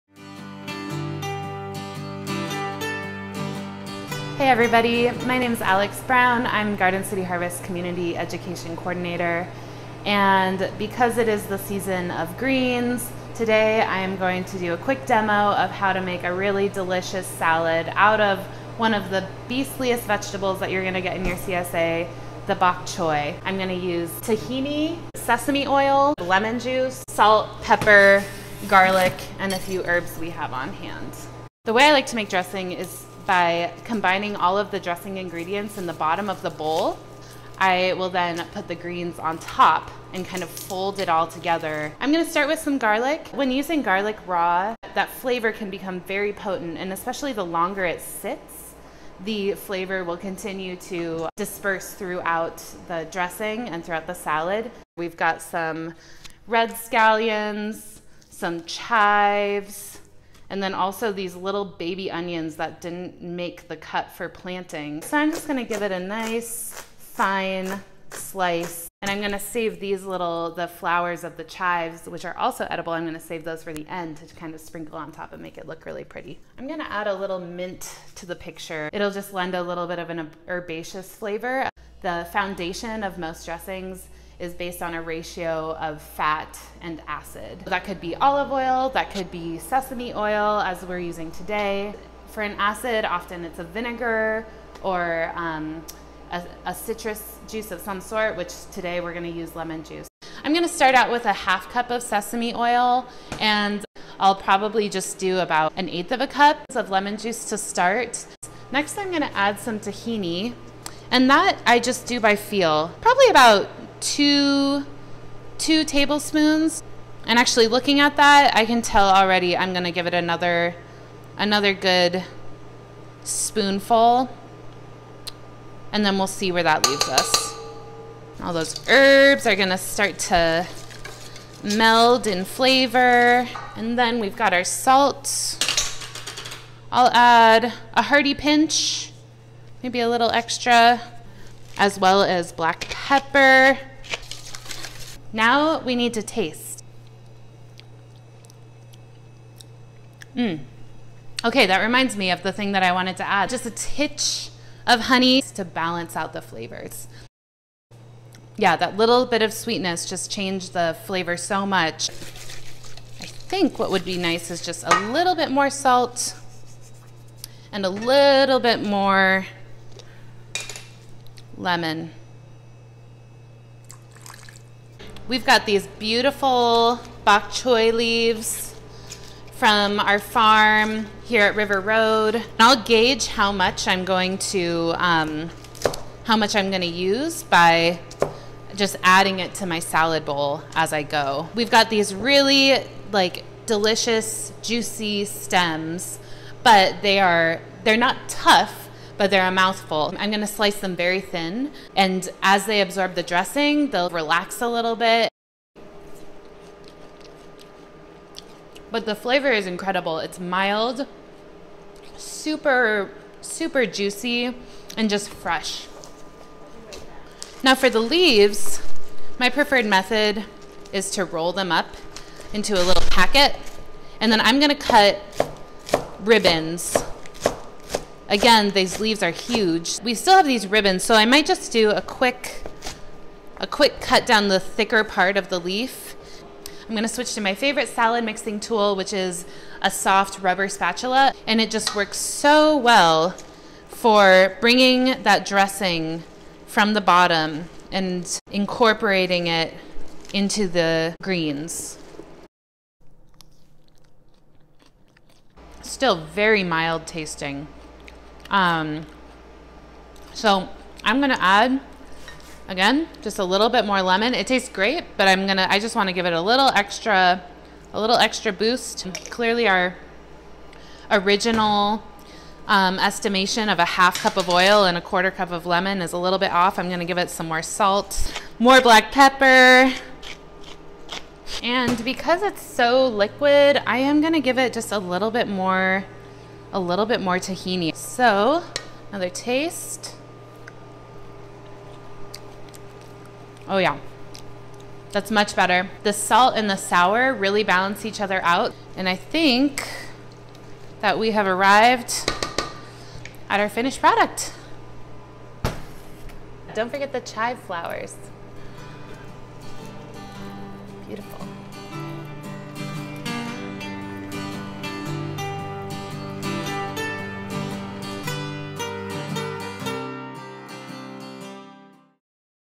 Salad Dressing: the basics +
From the basic formula balancing acid and fat to shortcuts to making a salad, Alexandra Brown offers some great salad making tips in 5 min or less. Bonus! How to adjust your dressing if it doesn't have the punch you want.